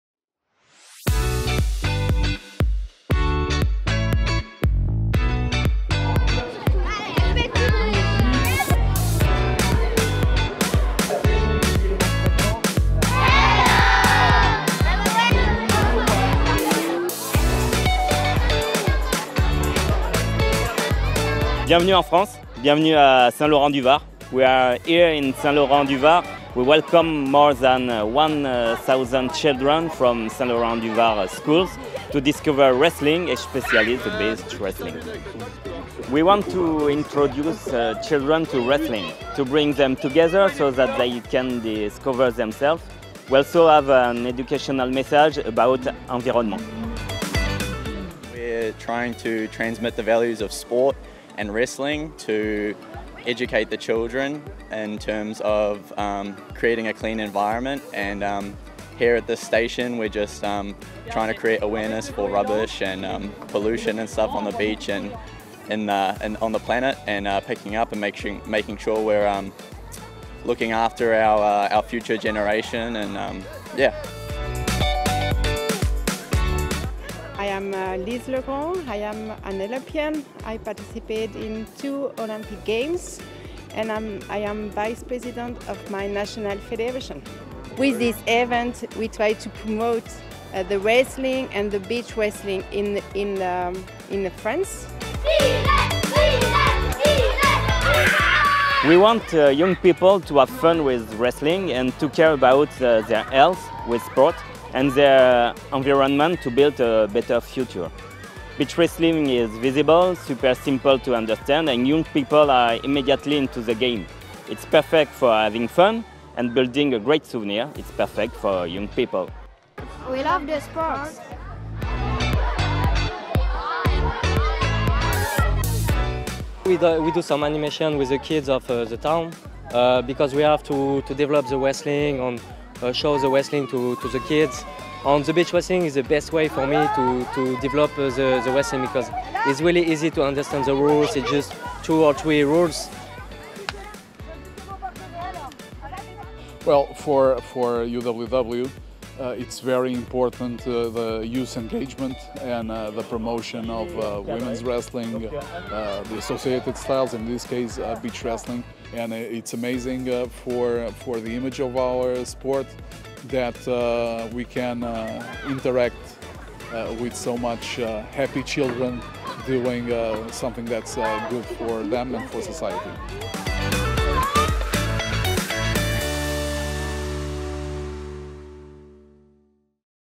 Back to School with Beach Wrestling 🤼‍♀️ 📚 🏖️
A thrilling beach wrestling day on Cousteau beach was even more exciting with the event doubling up as an attraction at the CAP 3000 Beach Sport Festival. French schoolchildren taking part in the national beach wrestling tour in conjunction with the Wrestling for Earth initiative came along to watch the action and take part in a little taster session themselves between the morning and afternoon sessions, adding to the fun of the day.